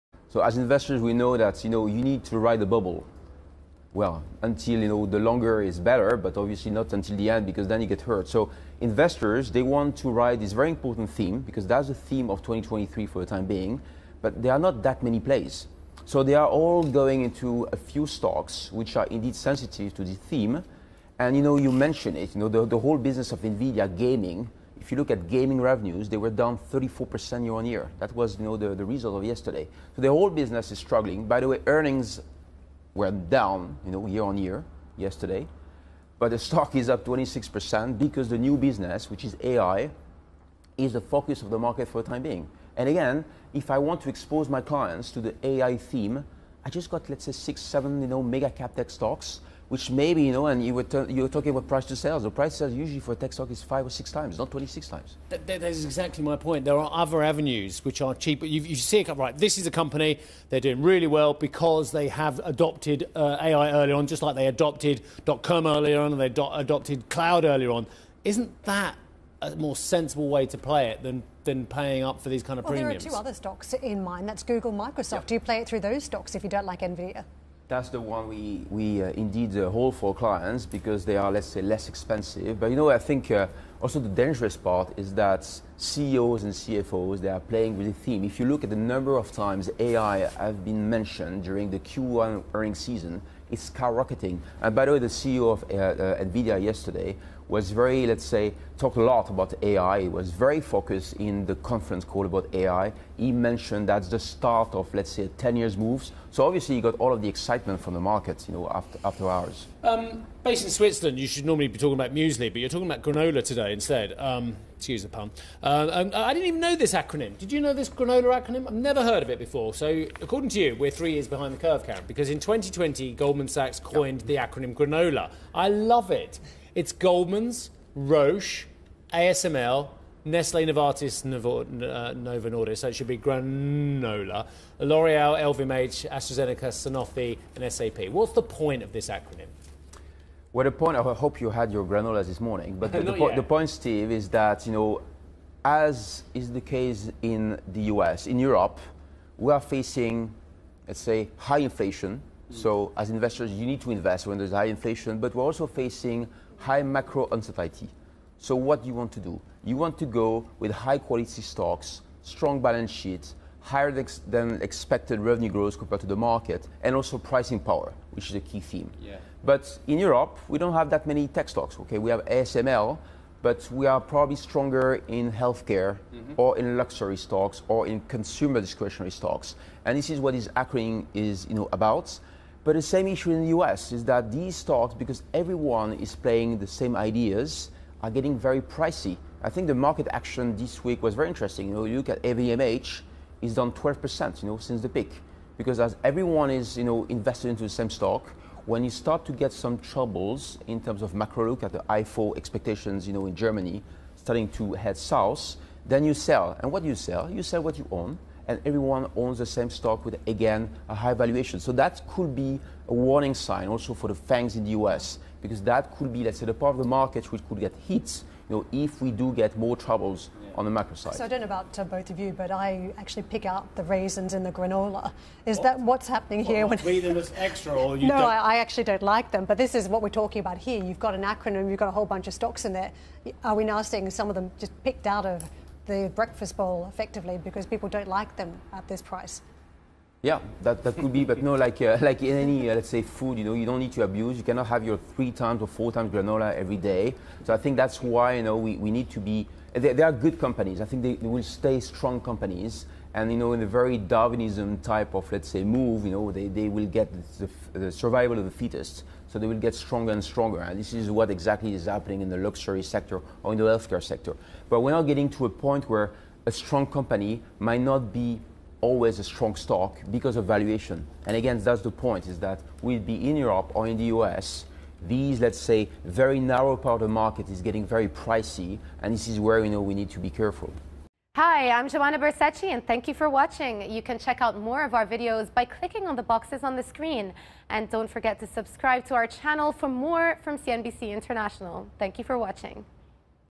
Investors interested in A.I. are all buying the same stocks, investment officer says
Charles-Henry Monchau, chief investment officer at Syz Group, discusses how investors are getting involved with artificial intelligence and how the market is becoming increasingly pricy.